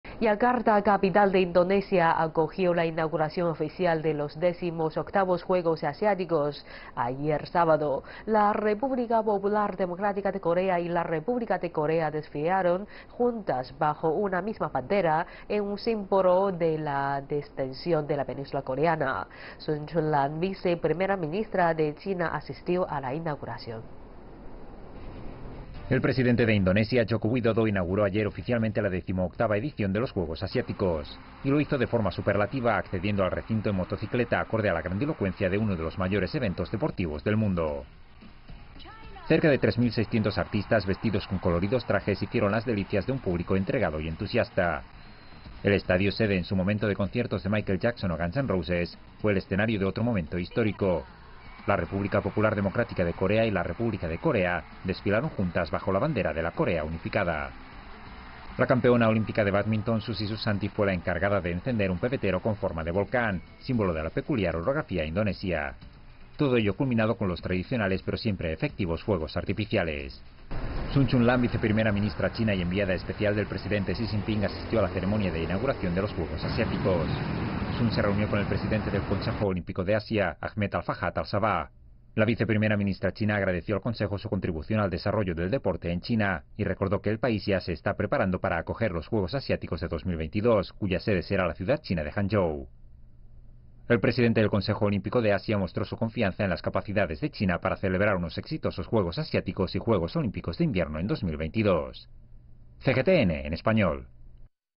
La República Popular Democrática de Corea y la República de Corea desfilan juntas
Yakarta, capital de Indonesia, acogió ayer la inauguración oficial de los decimoctavos Juegos Asiáticos. La República Popular Democrática de Corea y la República de Corea desfilaron juntas bajo una misma bandera, en un símbolo de la distensión en la Península Coreana. Sun Chunlan, vice primera ministra de China, asistió a la inauguración.